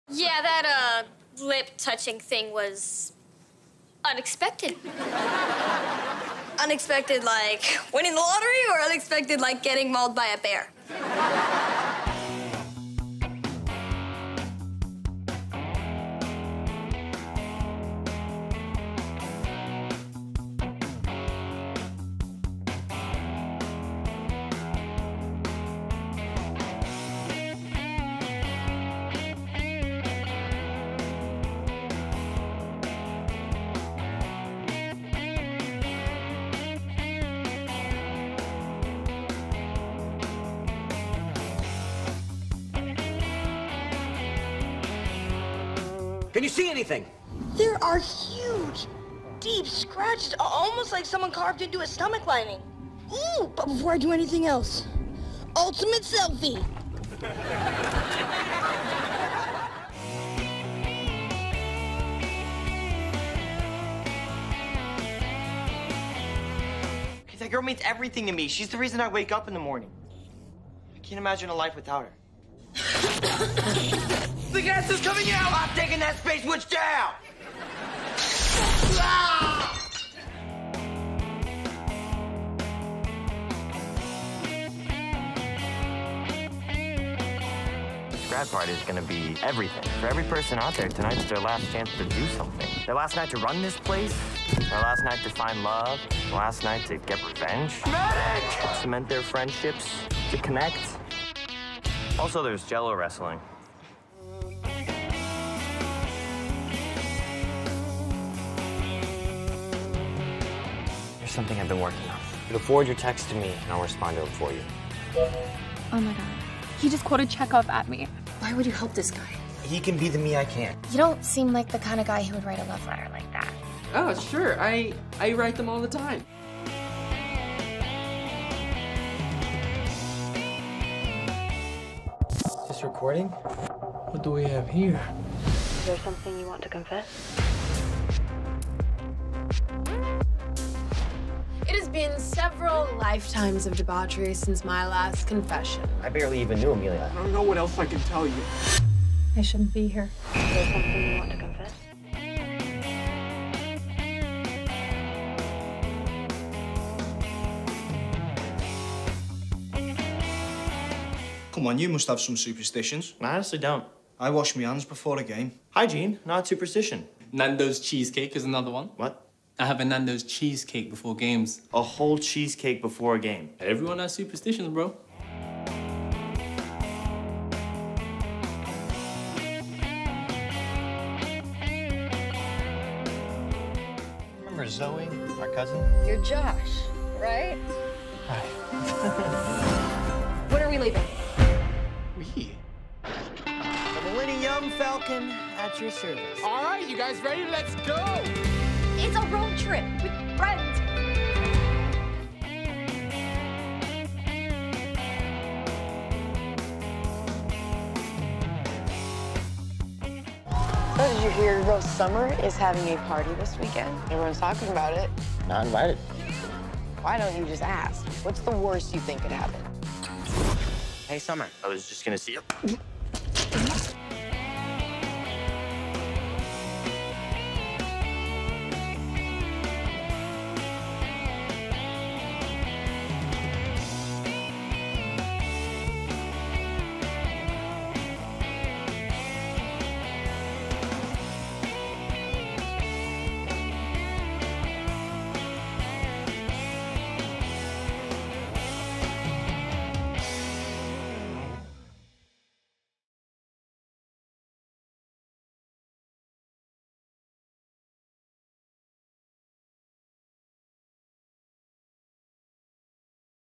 Where's Fletcher From A.N.T. Farm? See What Jake Short Is Up to Now
Jake Short (Fletcher Quimby) was 14-years-old when Disney Channel's A.N.T. Farm first aired. Now, he's 23 years old and has been in a ton of other movies and TV shows. What else is the actor doing now? We're breaking it all down for you in the video above. Enjoy!